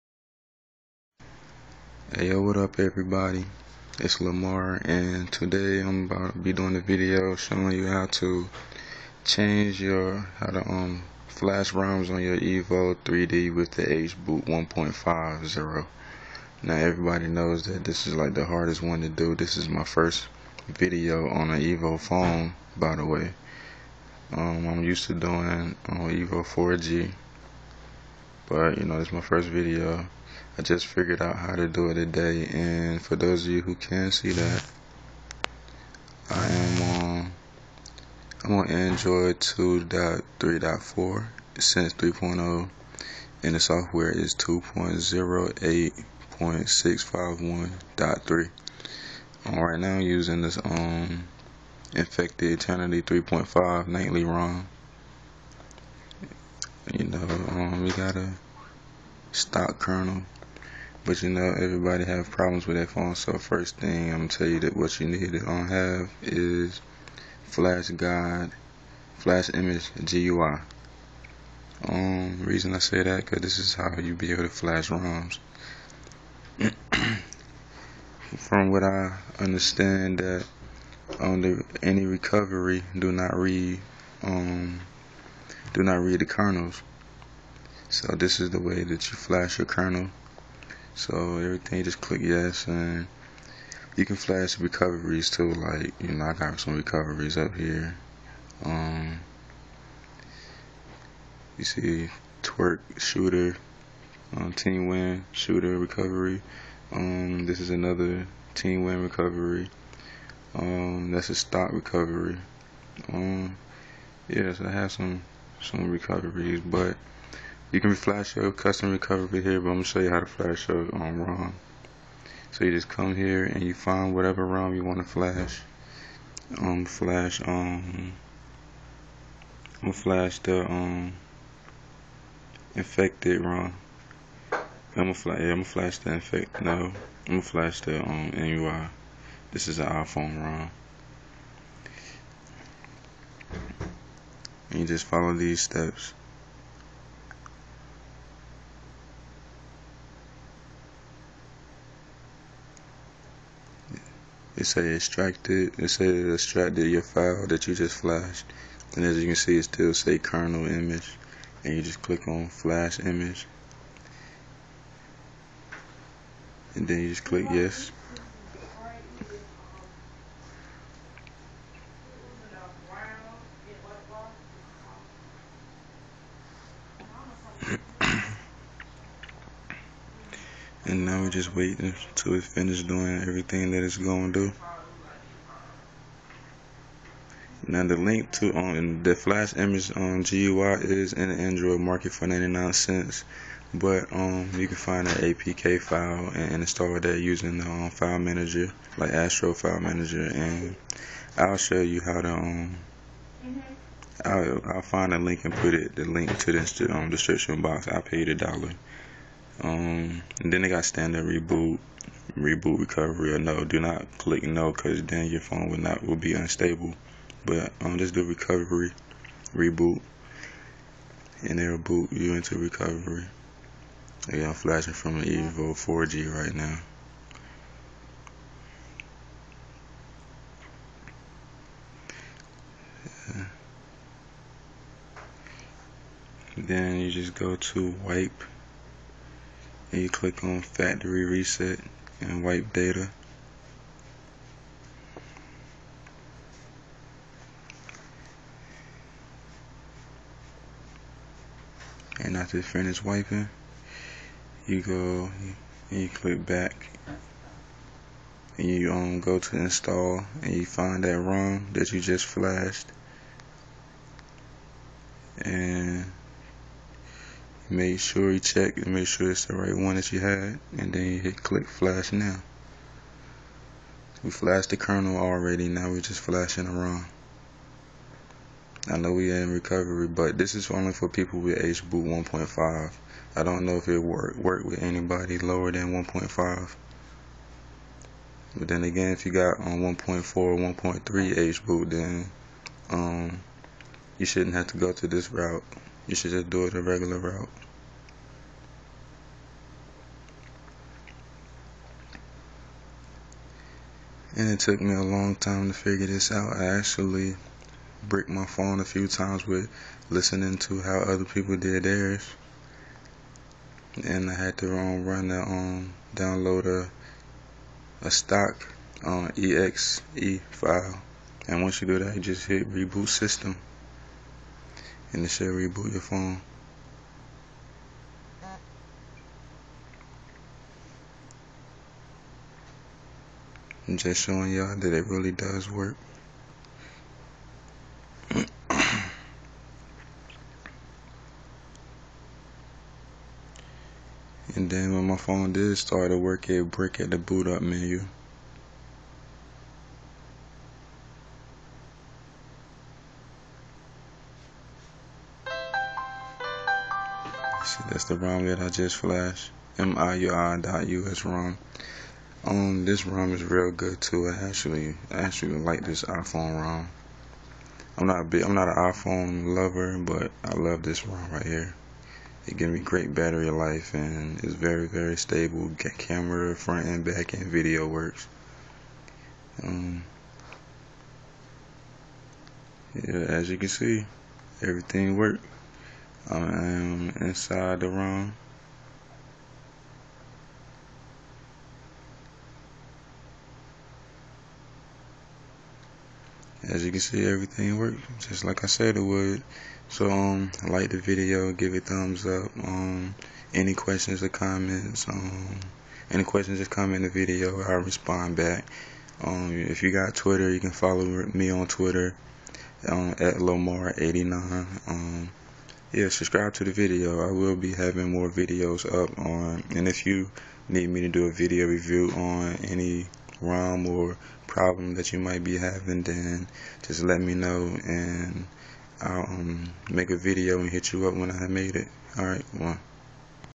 How To Flash Evo 3D Roms With Evo 3D Hboot 1.50
Must have Flash Image GUI from android market. its $1 or download apk file n install it using Astro File Manager from android market. Flash rom by finding it inside the Flash Image GUI app n then reboot recovery n flash the same rom again.